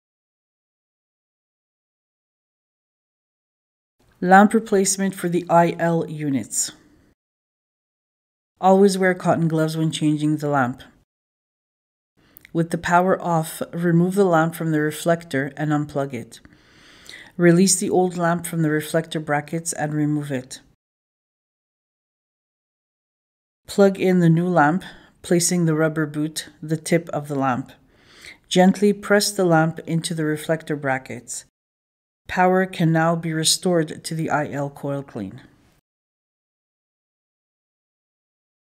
Lamp Replacement | IL Coil Clean
Here is how to replace the IL Coil Clean lamp.

Use only specified SANUVOX branded replacement UV lamps for your UV unit or system. Use of any other lamps will void your warranty and can result in damage to the UV unit or system.

BE SURE TO RESET YOUR UNIT AFTER CHANGING LAMP(S).

For more information on this product or to order replacement lamps,